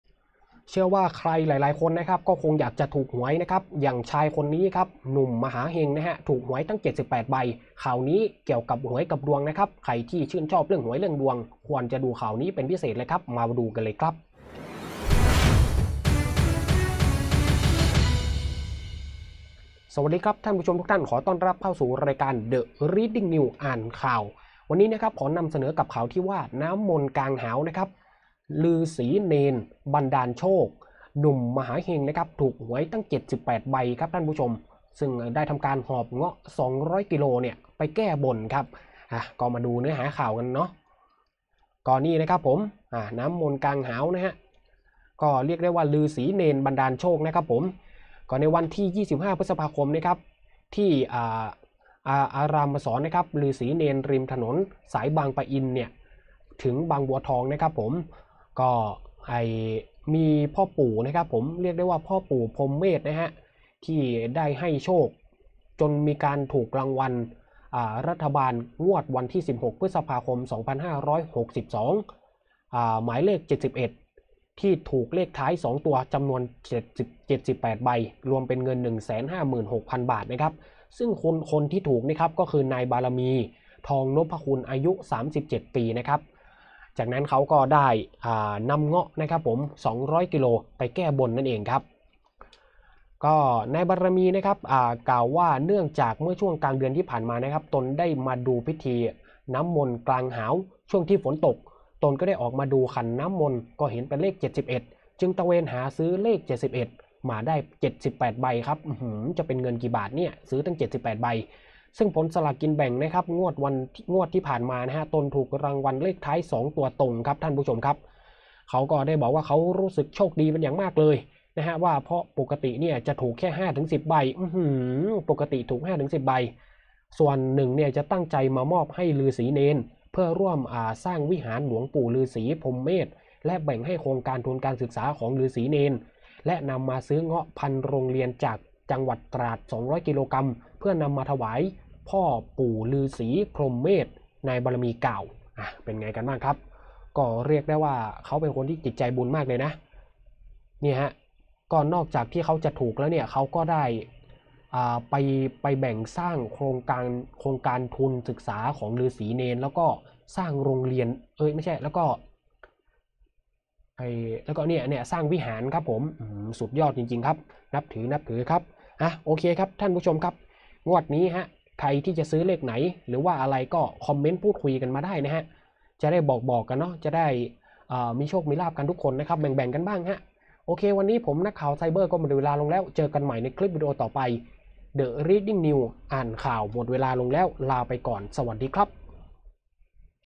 น้ำมนต์กลางหาว ฤาษีเณร บันดาลโชค หนุ่มมหาเฮงถูกหวย 78ใบ หอบเงาะ 200โล แก้บน
ขอต้อนรับเข้าสู่ช่อง TRNอ่านข่าว

วันที่ 25 พ.ค. ที่อาศรมฤๅษีเณร ริมถนนสายบางปะอิน-บางบัวทอง ต.โพแตง อ.บางไทร จ.พระนครศรีอยุธยา นายบารมี ทองนพคุณ อายุ 37 ปี ได้นำเงาะ 200 กิโลกรัม มาแก้บนพ่อปู่ฤๅษีพรหมเมศหลังจากที่ได้รับโชคถูกสลากกินแบ่งรัฐบาลงวดวันที่ 16 พ.ค.2562 หมายเลข 71 ที่ถูกเลขท้าย 2 ตัว จำนวน 78 ใบ เป็นเงิน 156,000 บาท

รับชม ข่าวสารใหม่ๆ ที่ช่อง TRNอ่านข่าว - TV